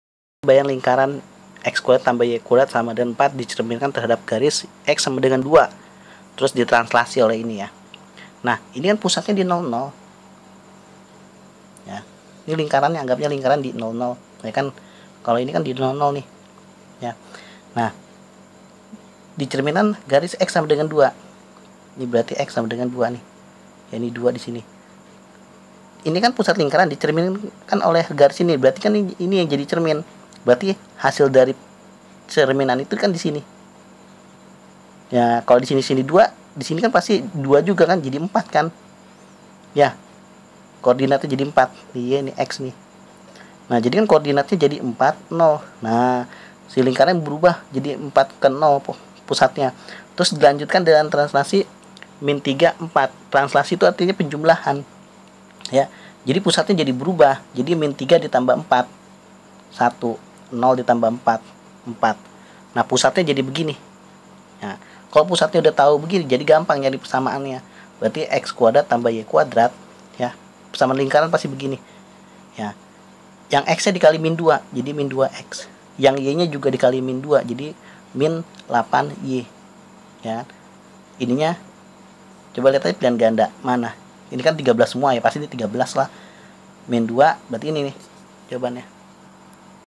Transformasi komposisi-Pembahasan soal UN Matematika IPA 2014
Transformasi komposisi dengan cara  cepat dan mudah.
Pembahasan ujian nasional matematika IPA 2014